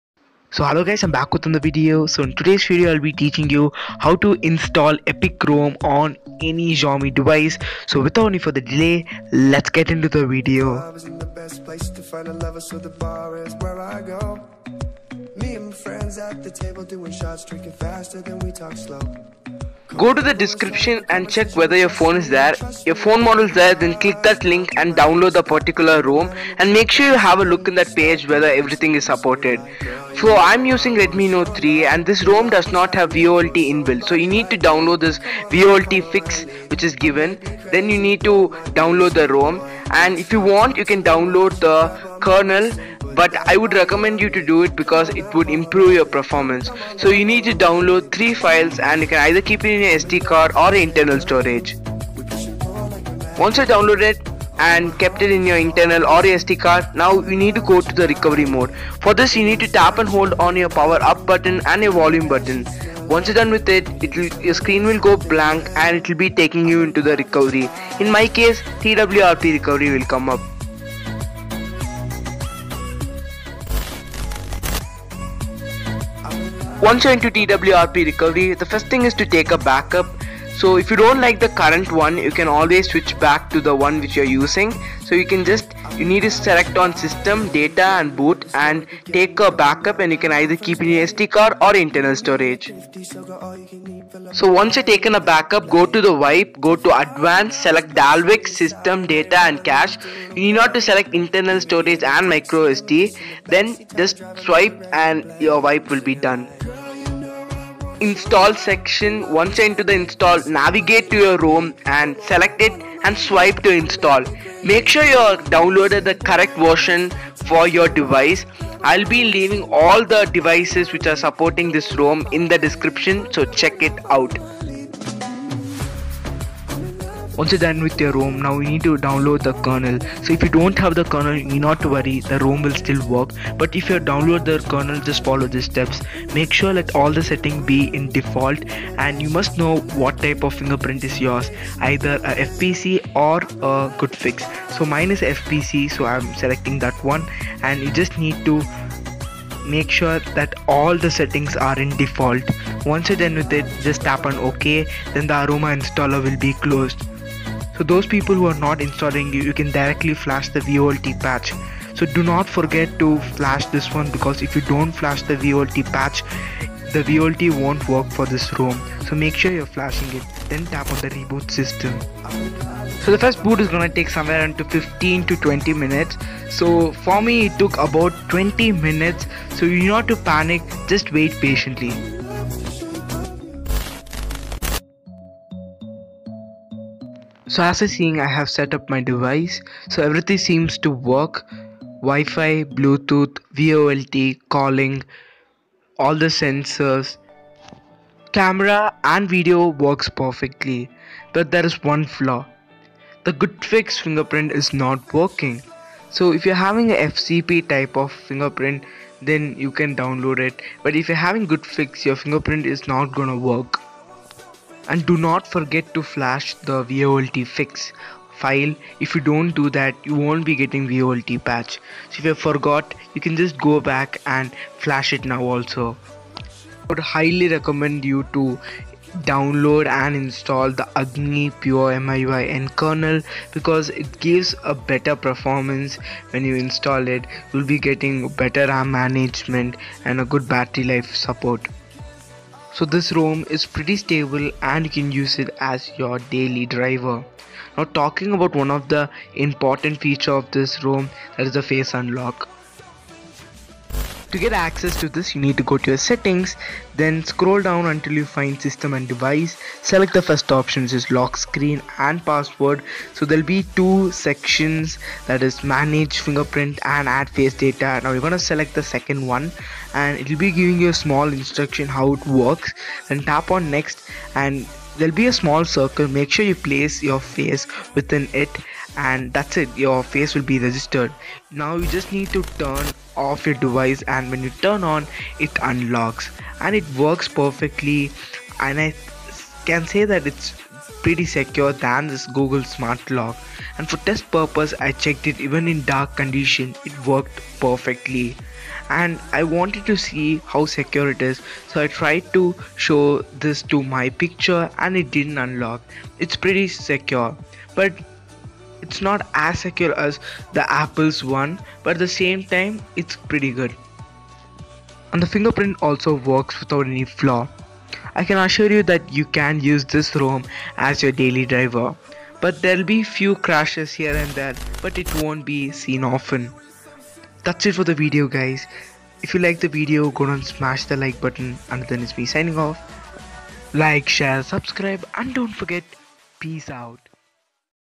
Install Epic ROM based on MIUI 9 with Face ID on any xiaomi device|| Nougat ( 7.0 )|Stable|VoLTE
Note - if u want Agni pure miui n kernel for your specific model then just search it up in Google you will get it

Install  Epic ROM based  MIUI 9 with Face ID on any xiaomi device|| Nougat ( 7.0 )|Stable|VoLTE

Hello guys I am back with another video in this video I will be showing how to Install MIUI 9 Epic ROM with Face ID on Redmi Note 3 || Nougat ( 7.0 ). It is a ROM based on Android 7.0 and it might get Android 7.1.2 update , even a Android Oreo (8.0) update who knows 😎. This ROM is stable and it has VOLTE , it is some time is buggy but most of the time it is pretty good . This ROM is face id or face unlock , with the help of this ROM you can face unlock on any Redmi device or xiaomi device

PEACE OUT

EpicRom, Miui, RedmiNote3, Kenzo, FaceID